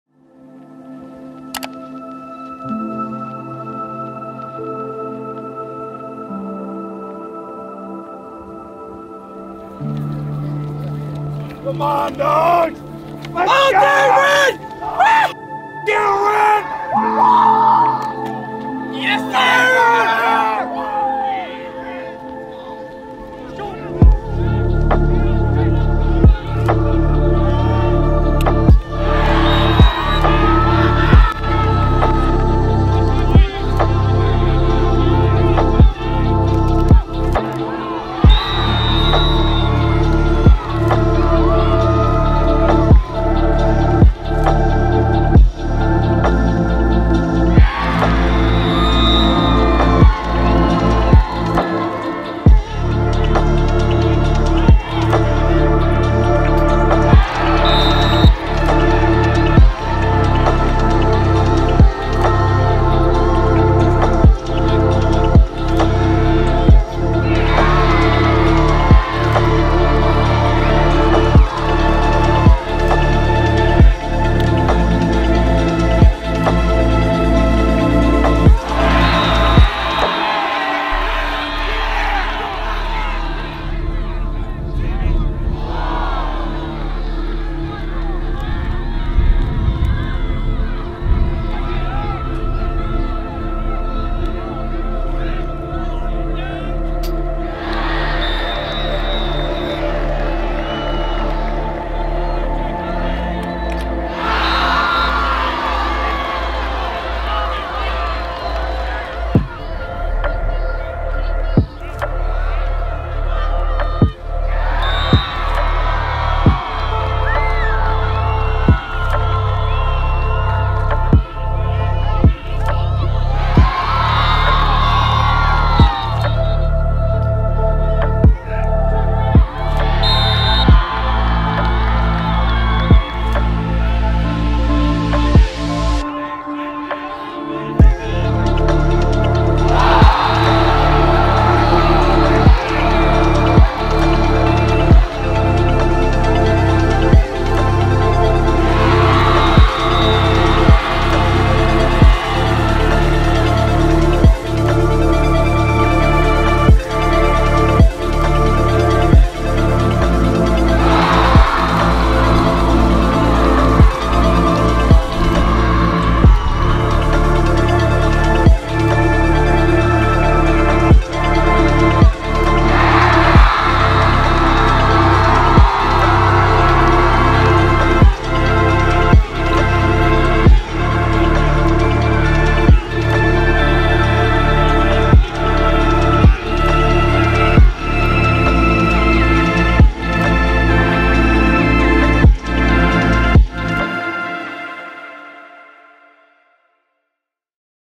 Boston University vs Harvard | 2022 College Highlights
It was a showdown in Massachusetts when #13 Boston University traveled to Cambridge to take on #15 Harvard. Led by a 5 point performance from Austin Madronic, Harvard came out on top with a 13-10 victory.